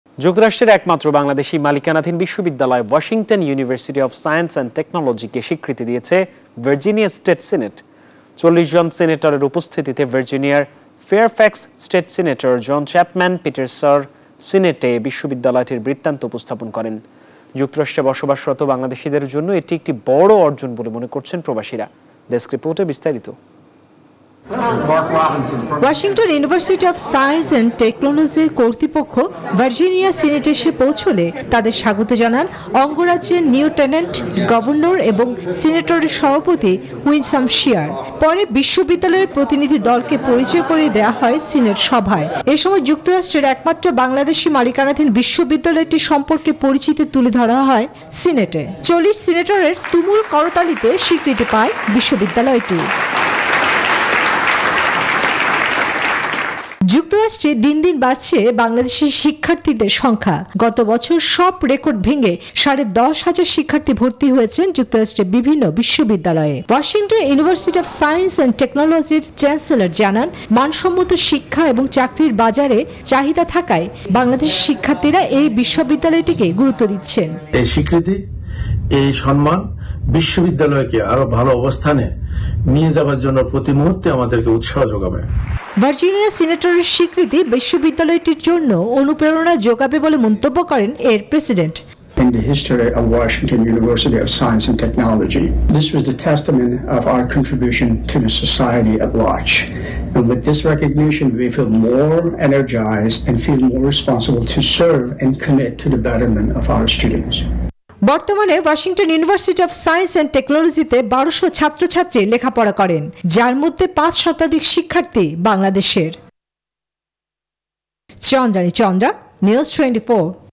যুক্তরাষ্ট্রের একমাত্র বাংলাদেশী মালিকানাধীন বিশ্ববিদ্যালয় | USA | News24
গত বছর সব রেকর্ড ভেঙে সাড়ে দশ হাজার শিক্ষার্থী ভর্তি হয়েছেন যুক্তরাষ্ট্রের বিভিন্ন বিশ্ববিদ্যালয়ে। ওয়াশিংটন ইউনিভার্সিটি অব সায়েন্স এন্ড টেকনোলোজির চ্যান্সেলর জানান, মান সম্মত শিক্ষা এবং চাকরি বাজারে চাহিদা থাকায় বাংলাদেশী শিক্ষার্থীরা এই বিশ্ববিদ্যালয়টিকে গুরুত্ব দিচ্ছেন।
Our other YouTube channels :
News24 Official Address:
NEWS24
371/A
Bashundhara Road
Block – D
Bashundhara Residential Area
Dhaka - 1229